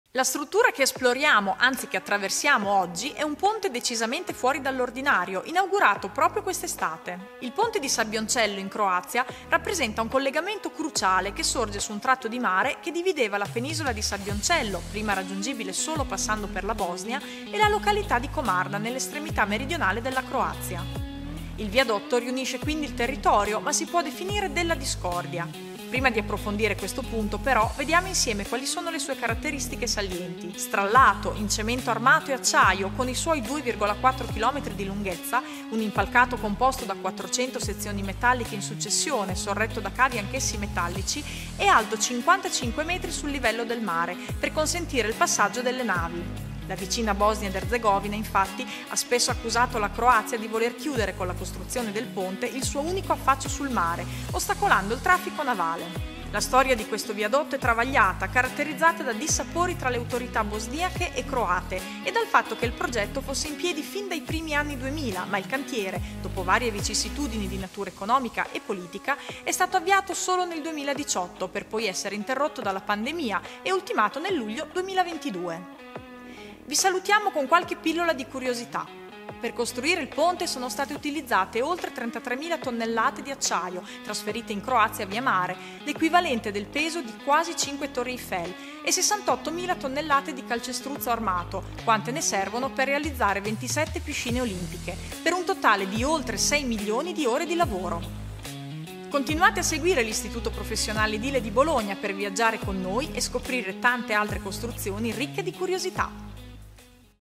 Ponte di Sabbioncello - Croazia
EDIFICI PARTICOLARI

Continuiamo a viaggiare insieme per il mondo!

Stavolta vi portiamo in Croazia, per visitare un ponte di freschissima inaugurazione. Dopo una storia travagliata durata anni, attraversando crisi economiche, politiche e sanitarie, nel luglio 2022 ha finalmente visto la luce il ponte di Sabbioncello, una struttura dal valore geografico davvero particolare.

Vediamo insieme perché.